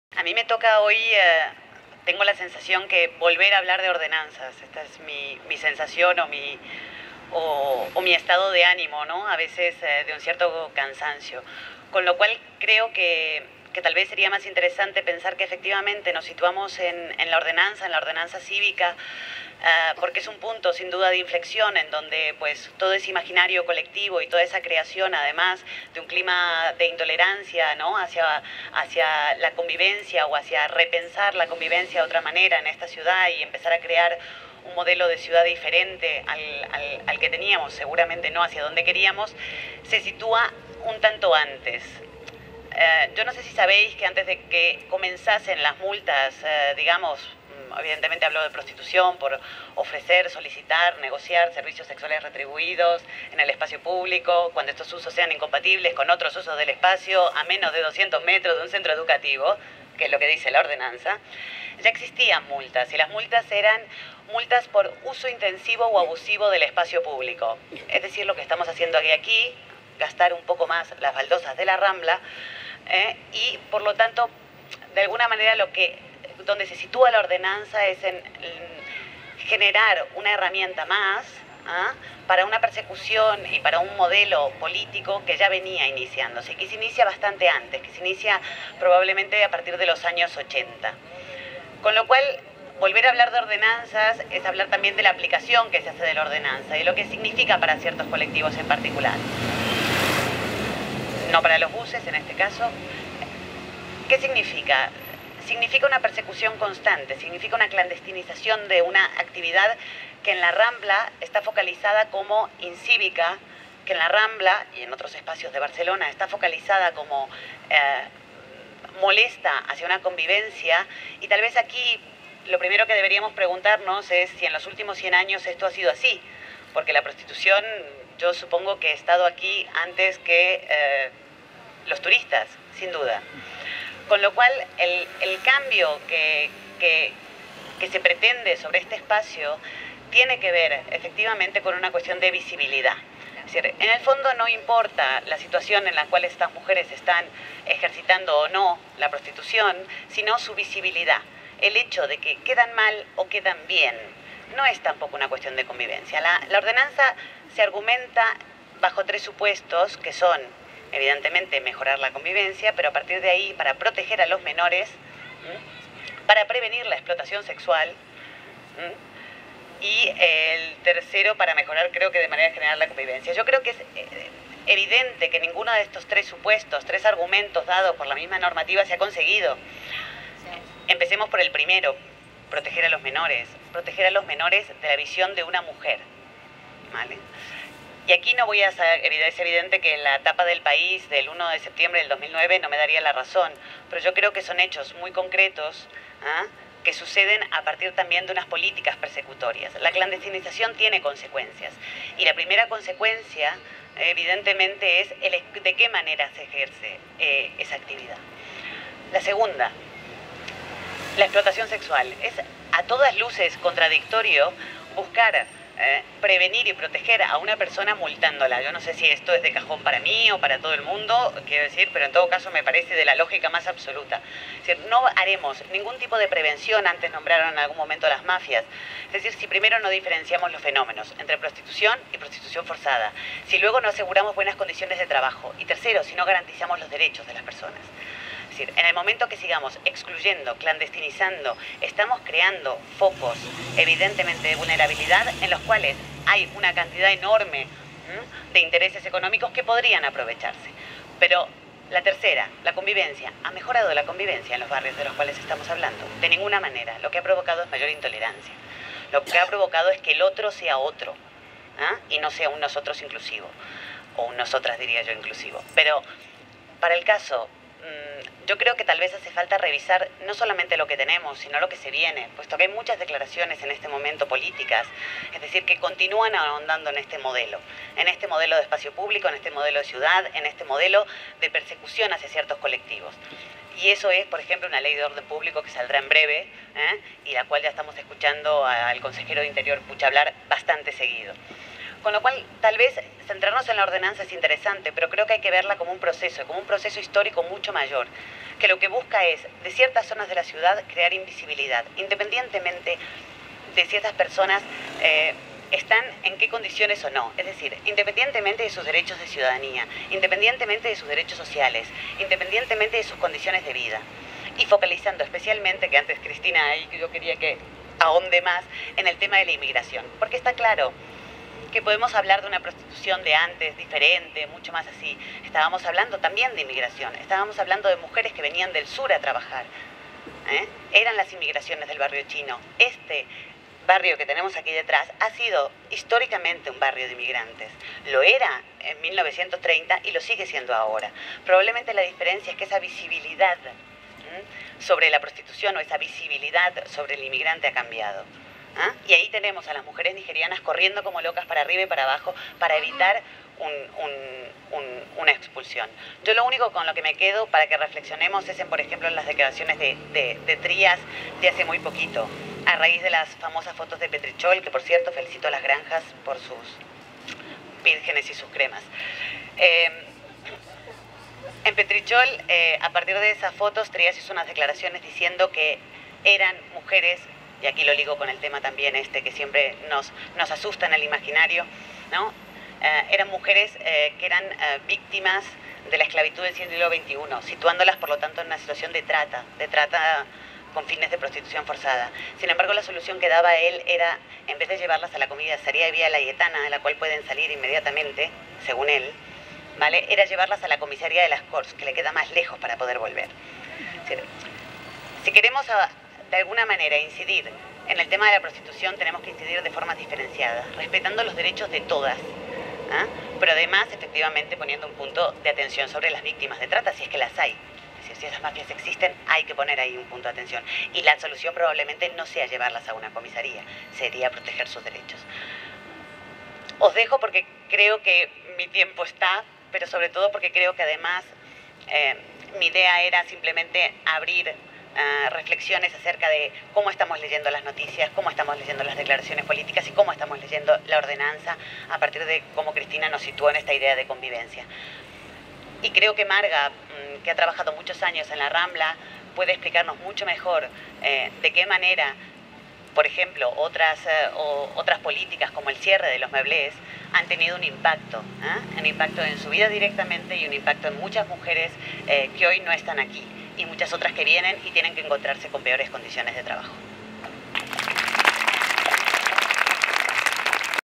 "Tornem a la Rambla"- Clarisa Velocci
V  - RAMBLA DE SANTA MÒNICA
... de quan els talons van deixar les seves marques en el marbre

LLoc: Rambla Santa Mònica (Davant les marques dels talons)
Tema: ordenança del civisme, usos de l'espai públic.

Clarisa Velocci (Activista de Genera, associació en defensa dels drets de les treballadores sexuals)